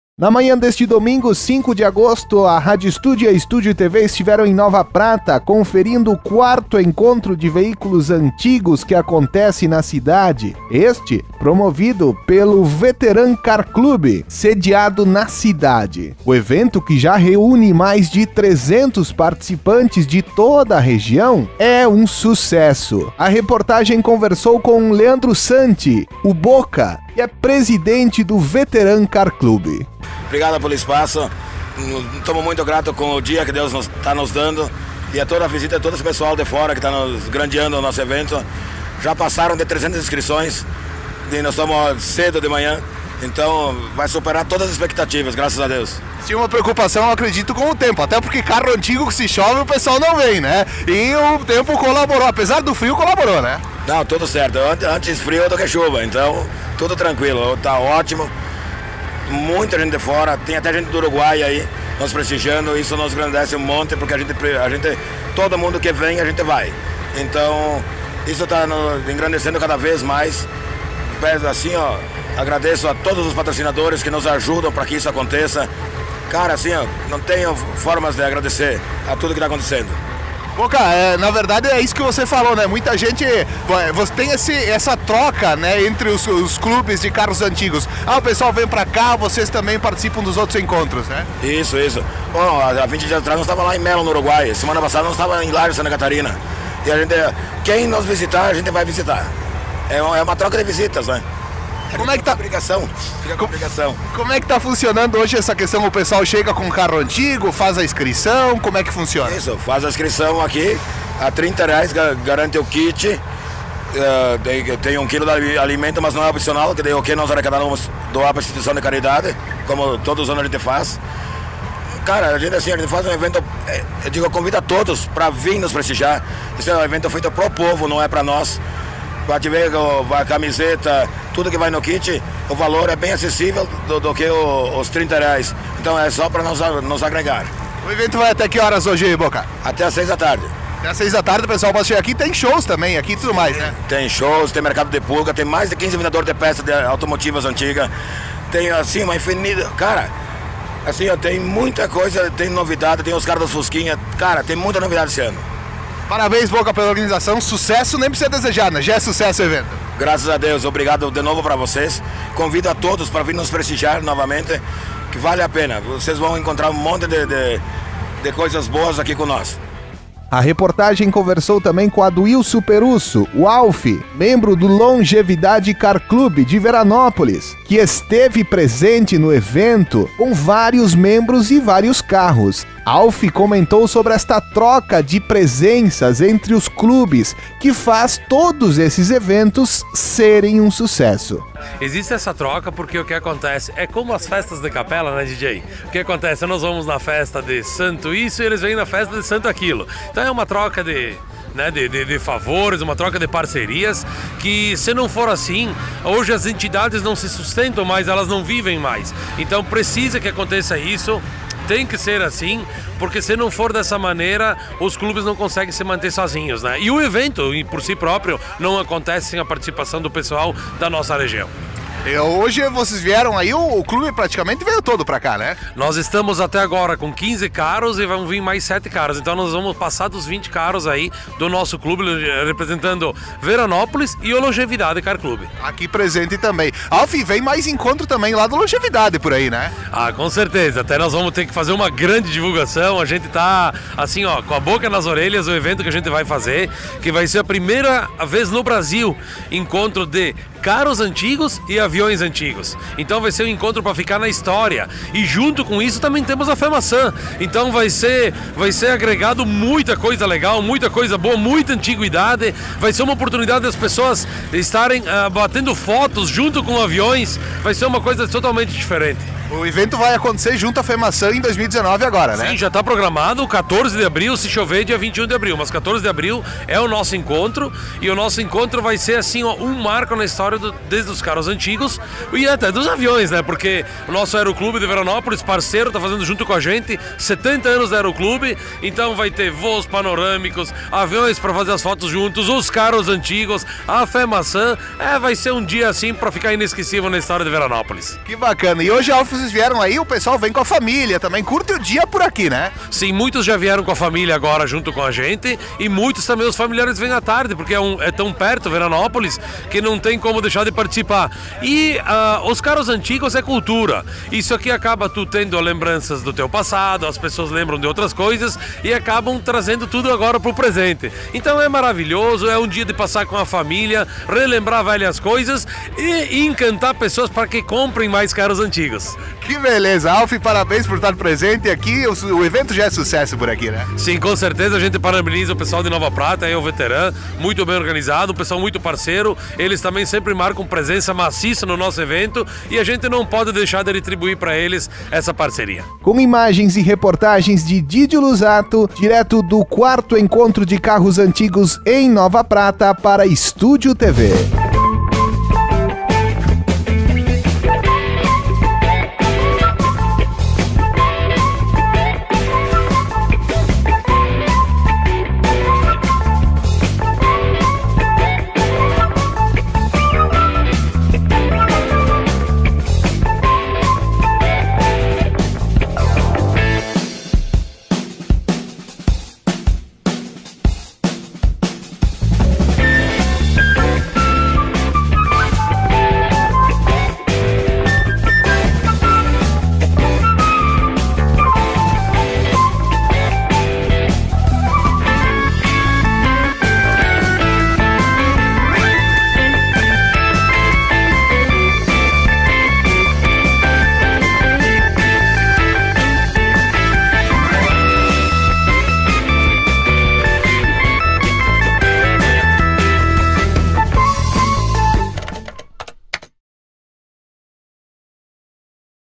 4.º Encontro de Carros Antigos de Nova Prata
StudioTV NovaPrata | Na manhã deste domingo, 05 de agosto, a Rádio Studio 87.7 FM e a Studio TV estiveram em Nova Prata conferindo o 4.º Encontro de Carros Antigos promovido pelo Veteran Car Clube, assista